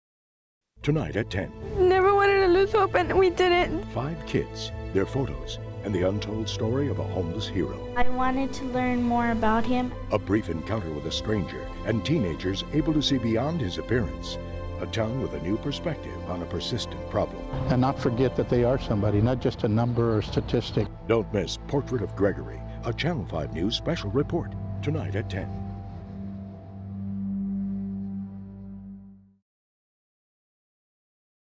Coming Up Tonight: Portrait of Gregory
Five students, their photos and the untold story of a homeless hero.